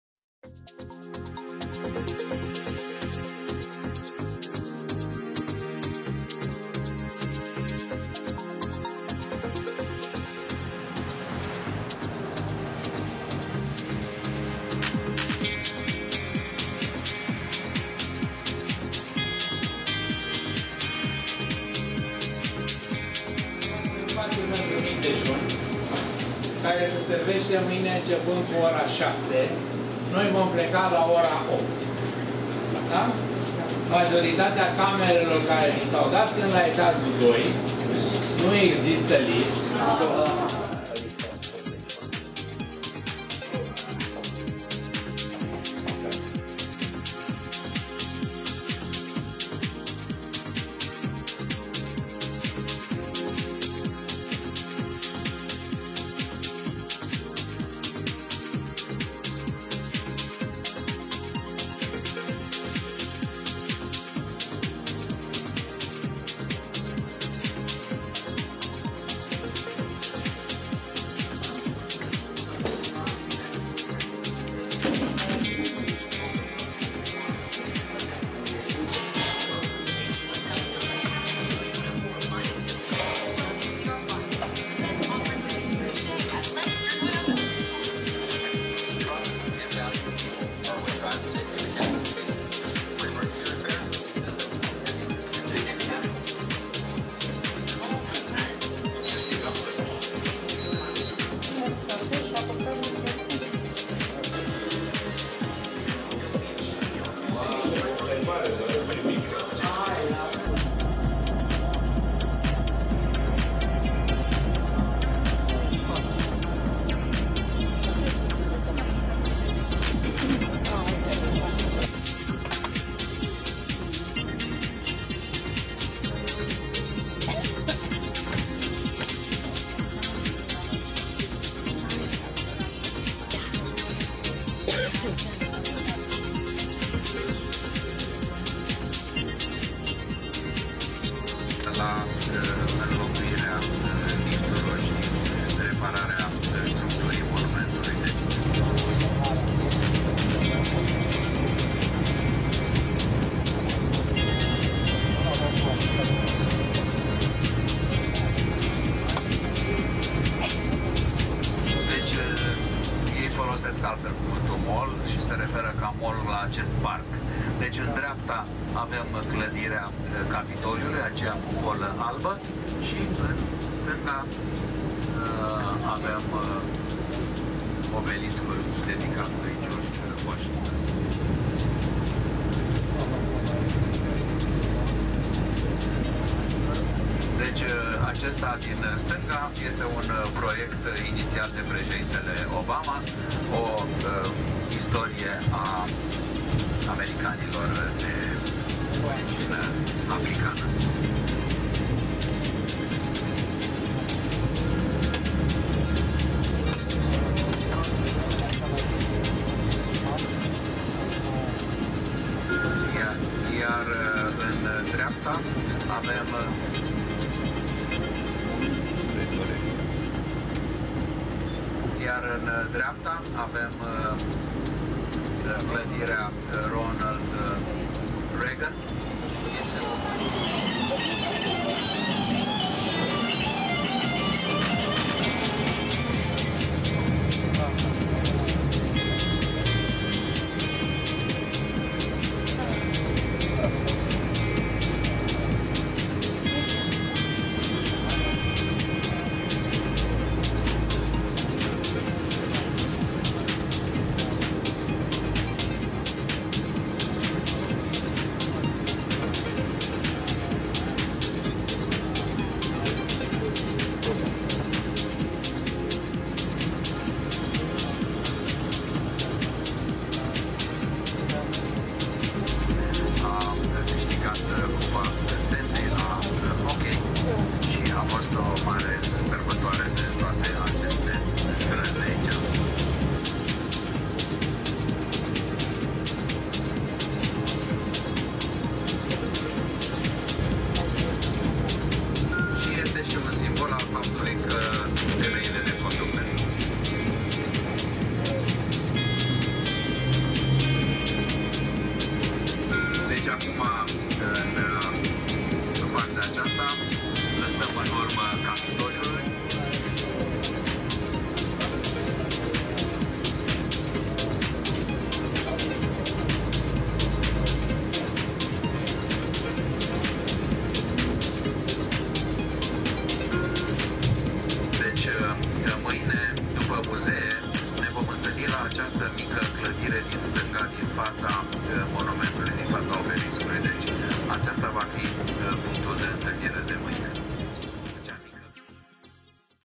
U.S.A.Bus tour in Washington DC - U.S.A. & Canada ep32 - Travel video vlog calatorii tourism
My trip to the U.S.A. and Canada through a travel agency
Călătoria mea în SUA și Canada prin intermediul unei agenții de turism